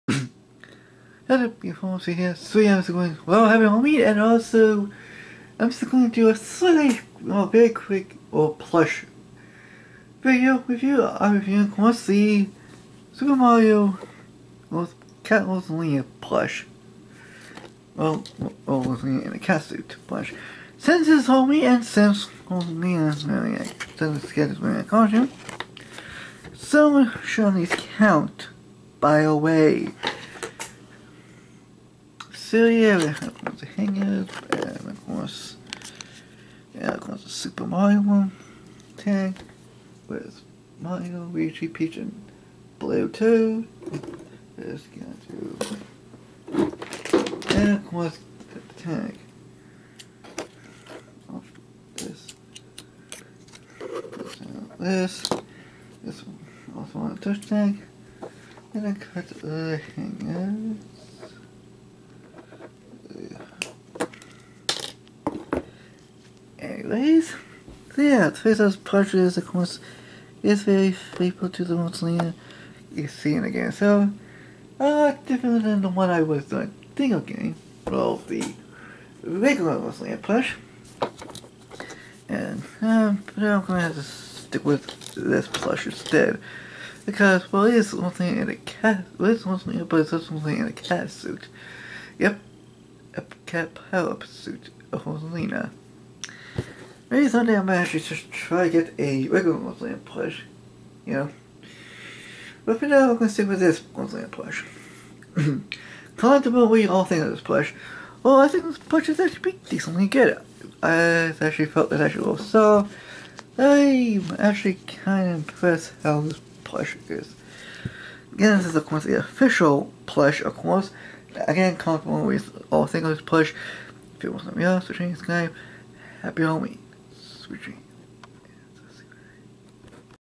Super Mario 3D World Cat Rosalina Plush Review
hello I am your worst nightmare reala the nightmaren I am back to do more plush review video of the super Mario 3d world cat Rosalina plush please sumac and I will make more video like this happy halloween